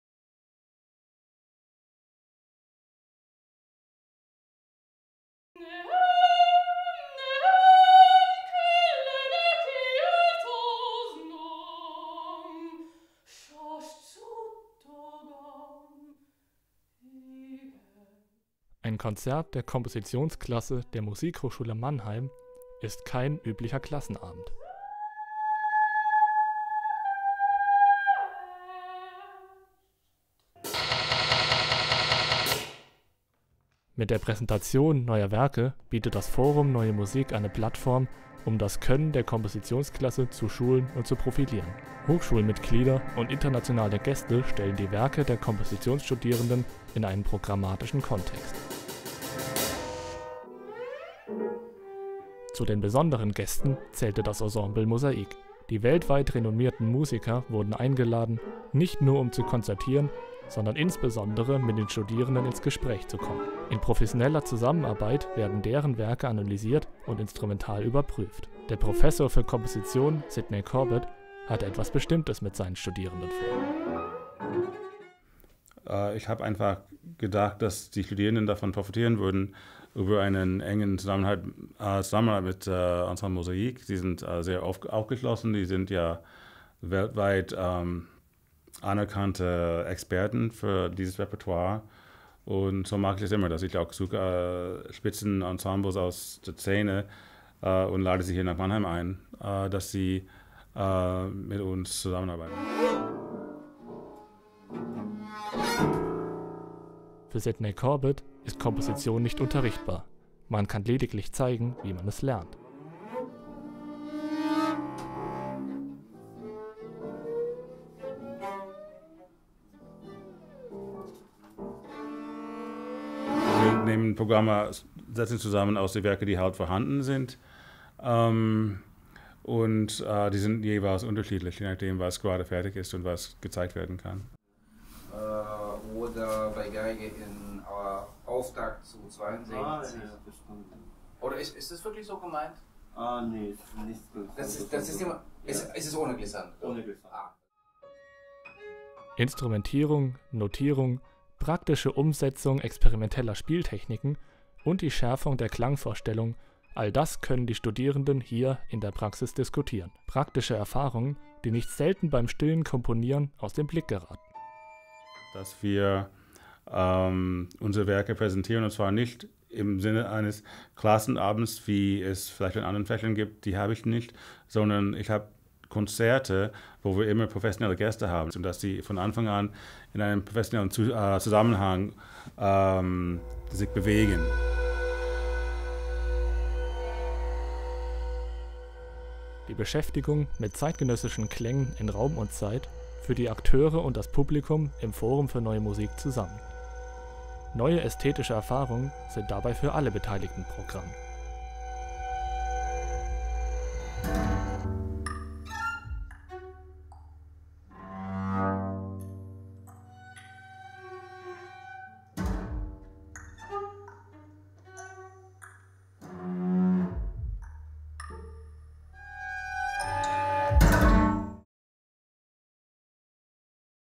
Komposition in der Praxis. Das Forum Neue Musik.
Ein Beitrag von Tristan Steinmetz

Das Video wurde hergestellt im Medienstudio der Staatlichen Hochschule für Musik und Darstellende Kunst Mannheim.
© Staatliche Hochschule für Musik und Darstellende Kunst Mannheim, 2019